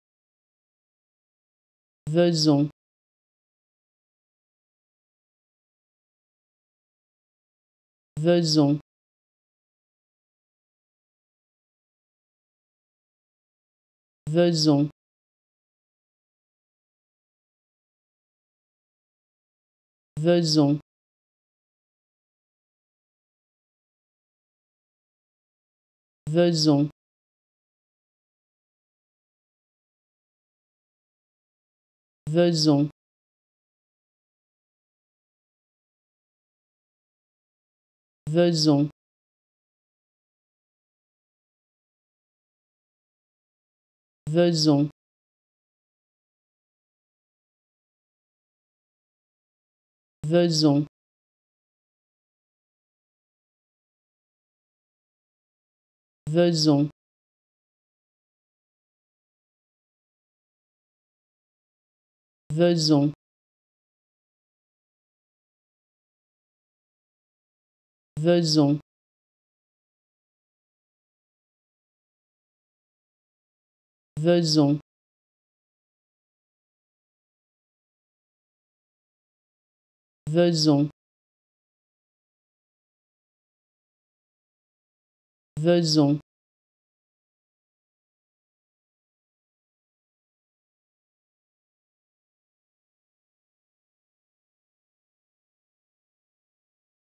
How to Pronounce ''Vezon'' Correctly in French
Learn how to say and properly pronounce ''Vezon'' in French with this free pronunciation tutorial.